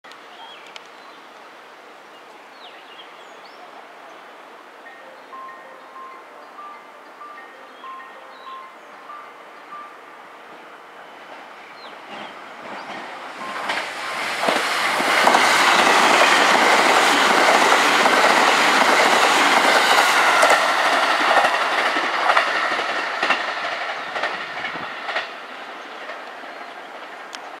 鳥取駅の接近メロディと同じものが使われているきのくに線・冷水浦駅
冷水浦駅です。
きのくに線ではこの駅だけ特殊な放送が流れます。
米子支社の接近メロディ・ほくほく線まつだい駅の接近メロディと同じものが使用されています。
特急くろしお１１号白浜行き通過